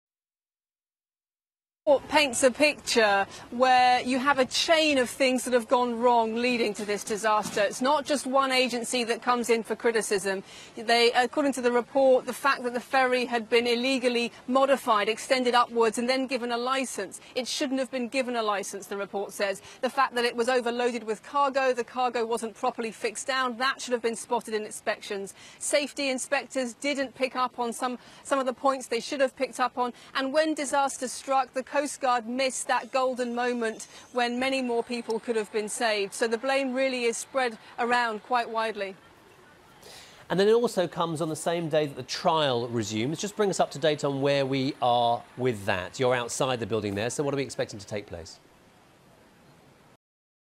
South Korea ferry 'sank due to negligence and corruption'
An interim South Korea government report says the 16 April sinking of the Sewol ferry was partly due to government negligence and corruption.

It cited lax regulation, poor safety inspections and a slow and badly-coordinated coast guard response as factors that led to the disaster.

Eleven officials have been referred to prosecutors on corruption charges and dozens more face possible legal action.